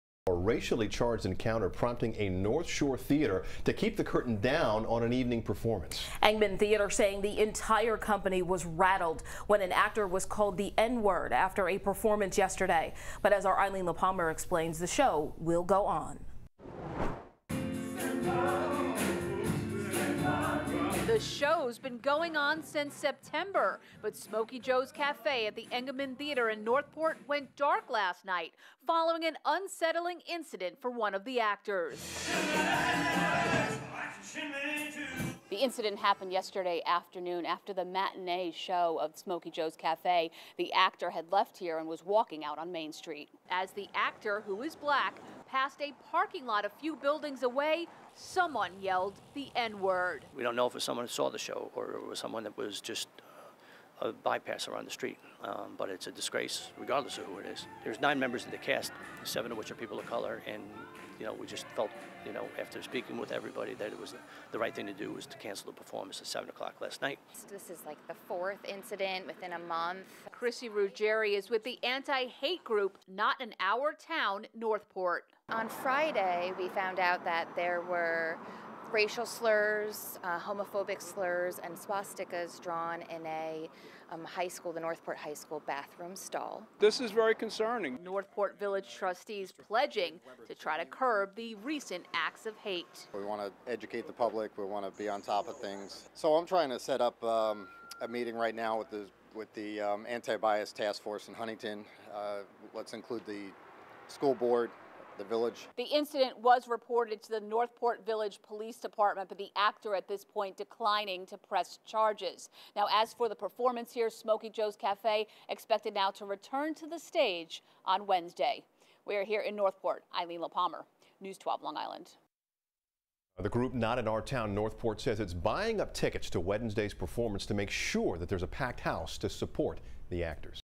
Racist incident rattles 'Smokey Joe's Cafe' cast at Engeman Theater in Northport
The co-owner of the theater Kevin J. O’Neill says the actors were very upset by the incident and decided to cancel the 7 p.m. performance of the show.